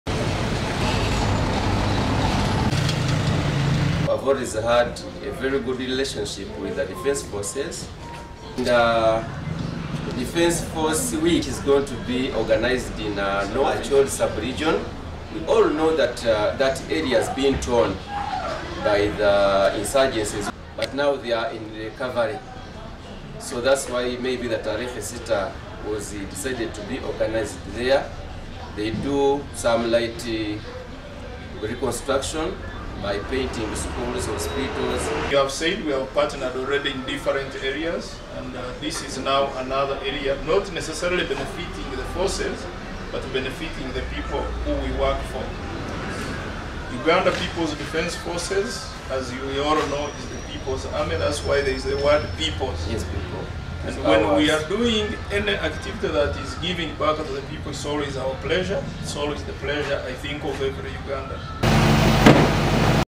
OLUNAKU LW’AMAGYE: Global Paints edduukiridde ebya Tarehe Sita
Entekateeka z’okujjukira olunaku lwa Tarehe sita  nga luno amagye ga NRA lwegalumba enkambi ye Kabamba zigenda mu maaso .

Ssabiti eddirira emikolo gino , amagye genyigira mu bintu ebyenjawulo omuli okuyamba abo abali obubi .

Wano Amyuka omwogezi w’amagye Lt Col Deo Akiiki wasiimidde Kampuni ya Global Paints evuddeyo okumakwatizaako nga ebawa laangi gyebagenda okukozesa mu bitundu bya Acholi okusiiga amalwaliro